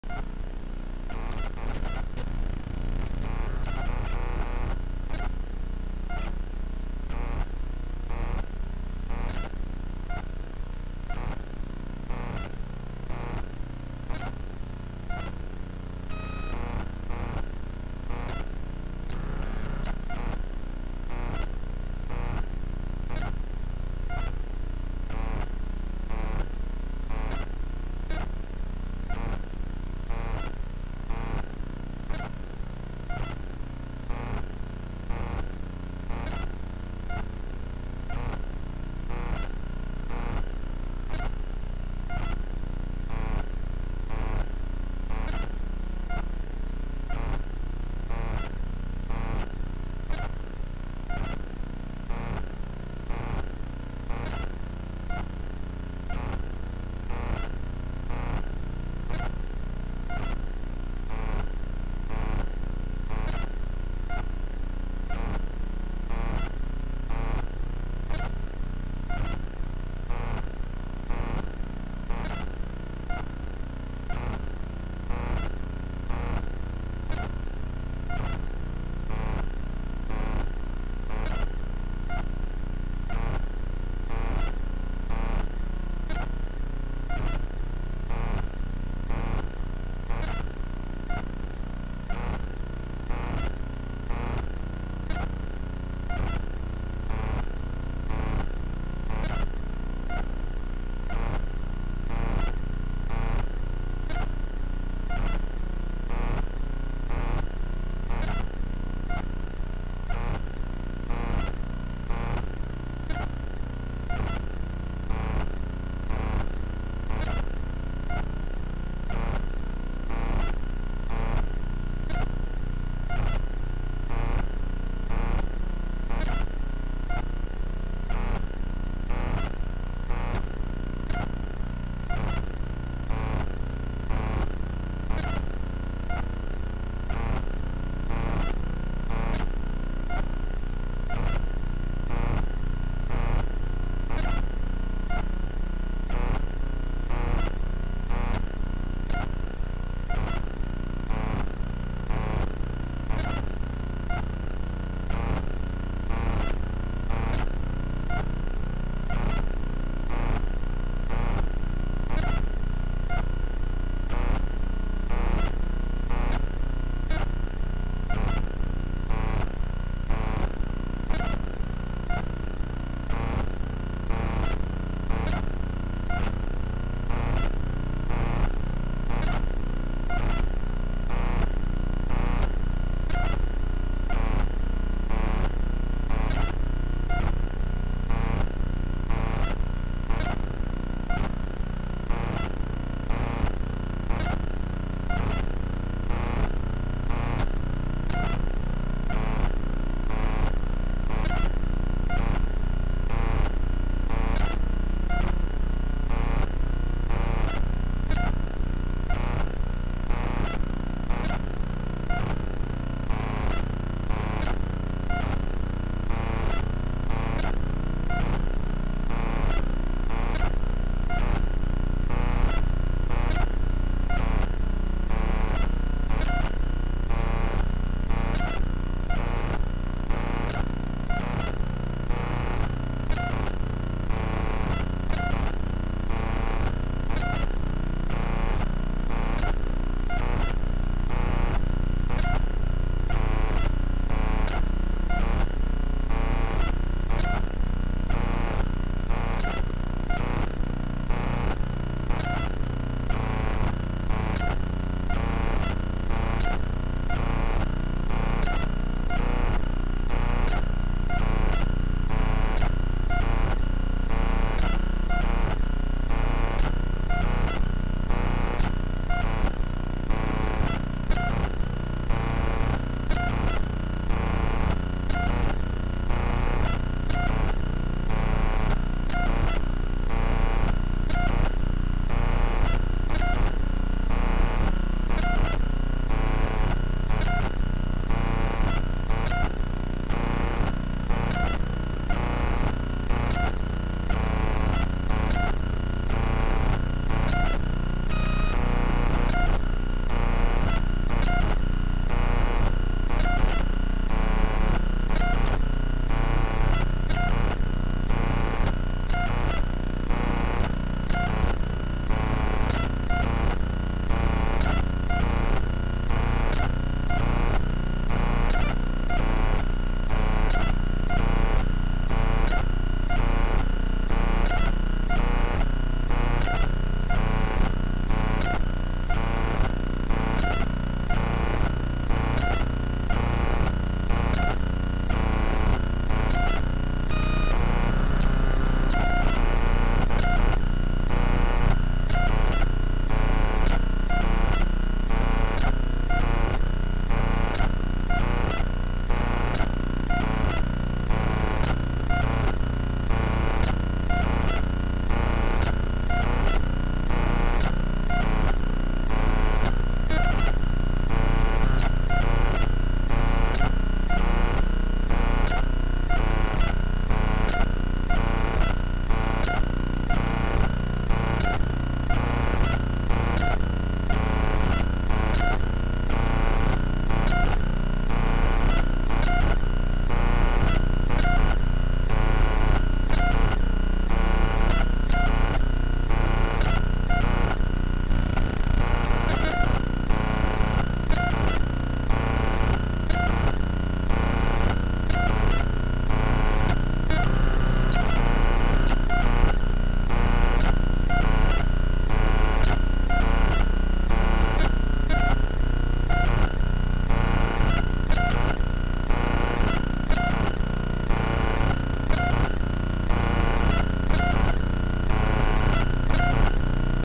Uszkodzenie kamerki
Uszkodzenie kamerki samochodowej - po tym nagraniu wypisuje komunikat "memory error", chociaż obraz na ekraniku jest prawidłowy i karta pamięci też działa.